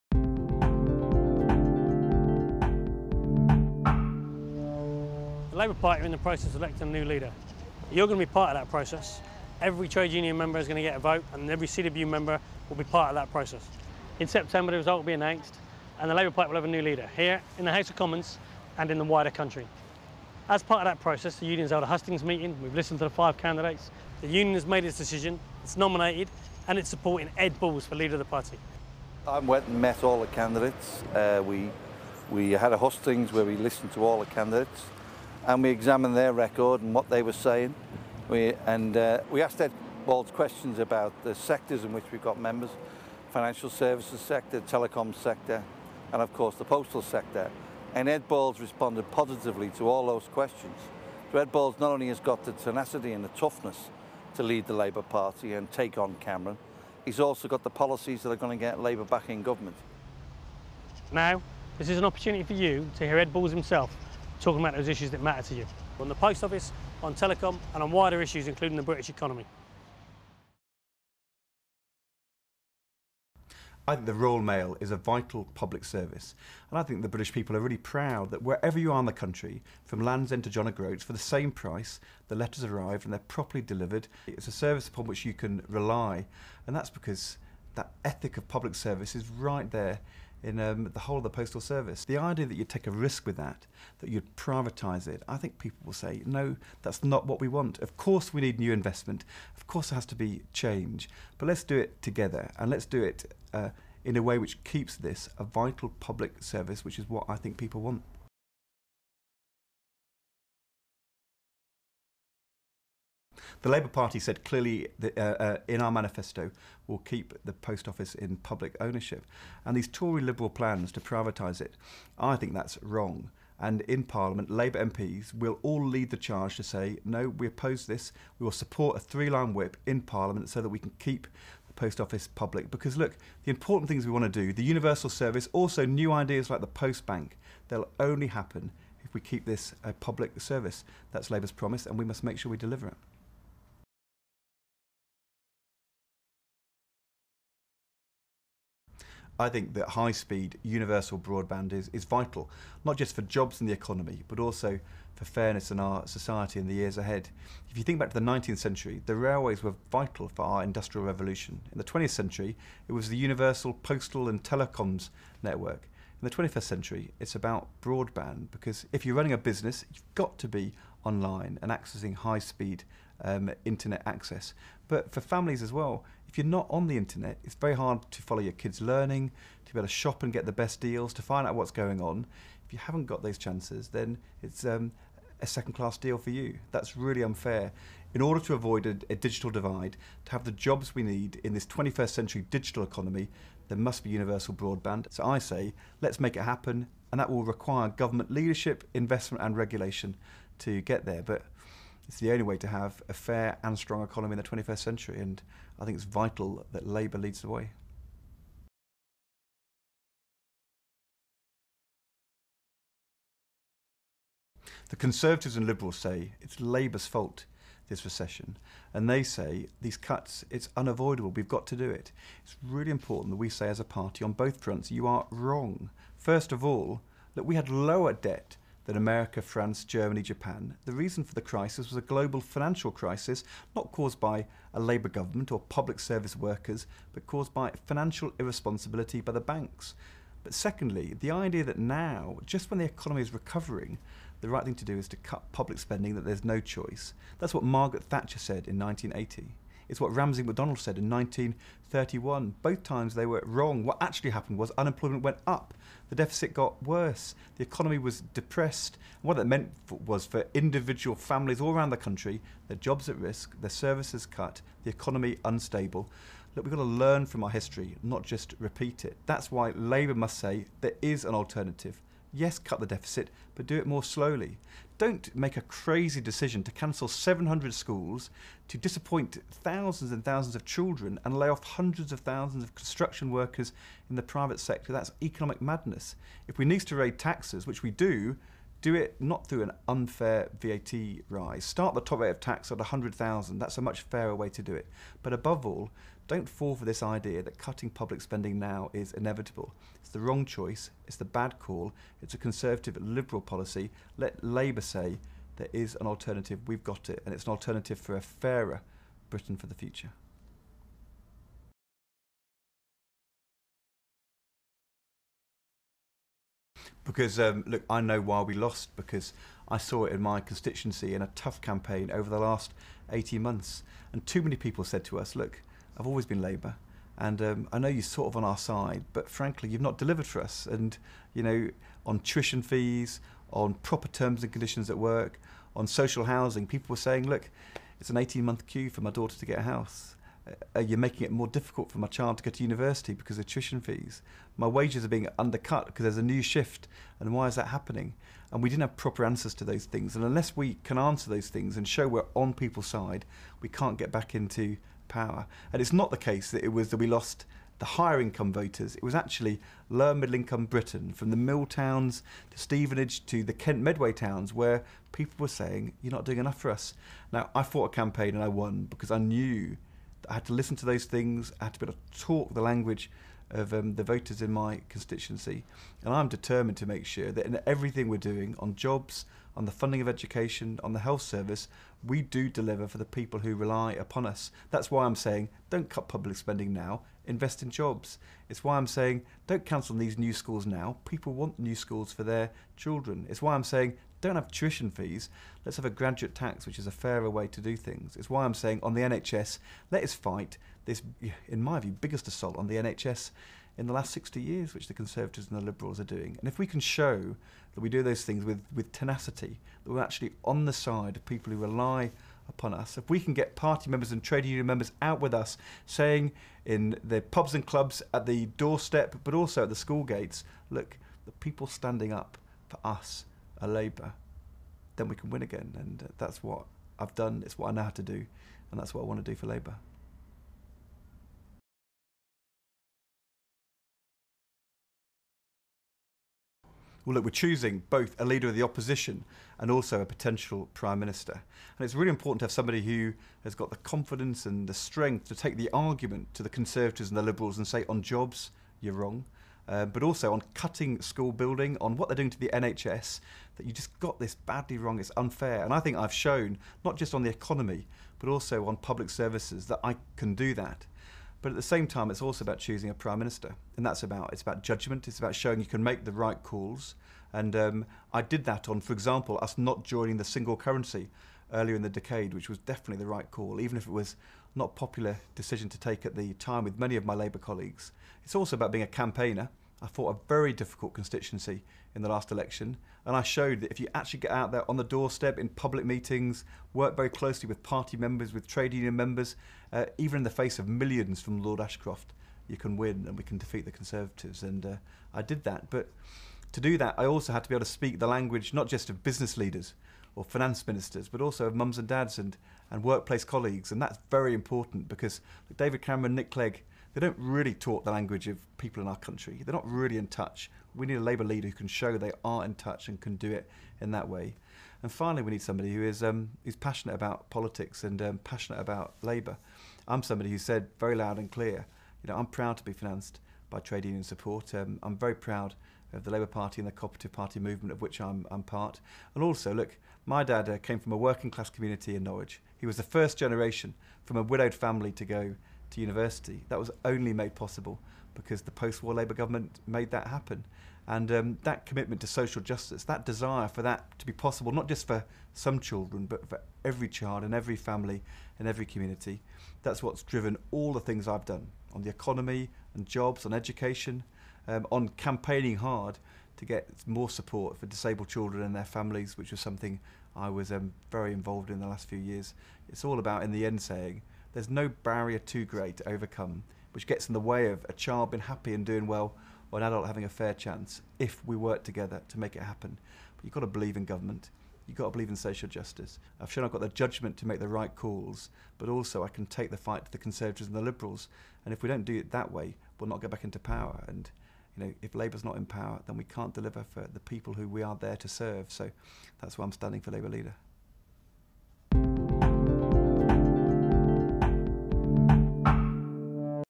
CWU TV Matters | Ed Balls Interview
CWU TV are granted an exclusive interview with the CWU nominated Labour Leadership candidate Ed Balls. In this in deapth discussion Ed Balls takes us through his plans for the future of the labour party and outlines his stance on key policies affecting both the CWU and the wider country. (Created August 2010)